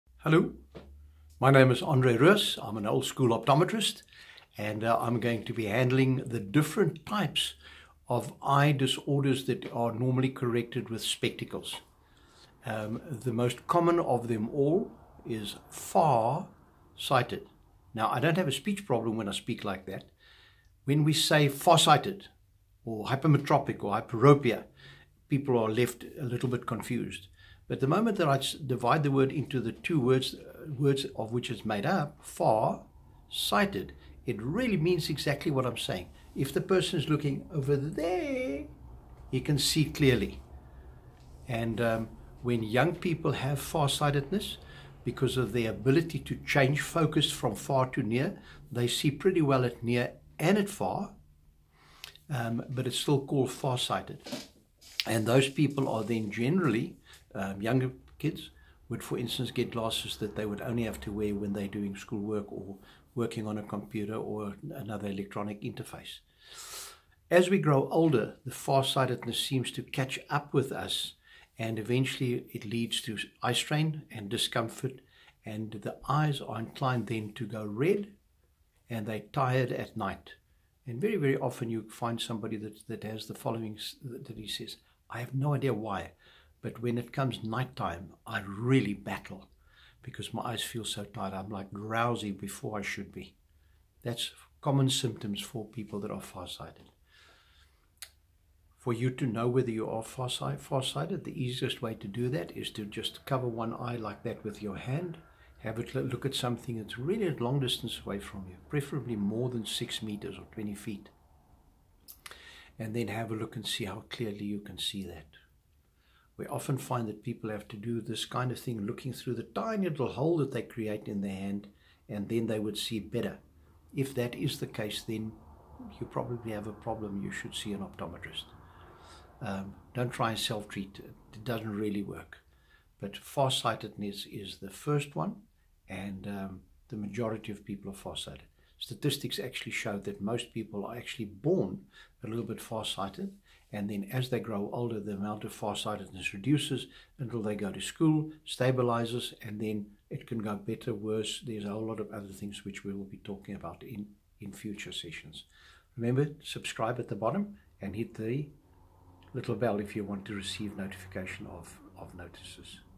EyeGuy's quick overview on FARsigted
Many people get really confused in understanding the differences between farsightedness and shortsightedness - In the very basic explanation farsightedness is discussed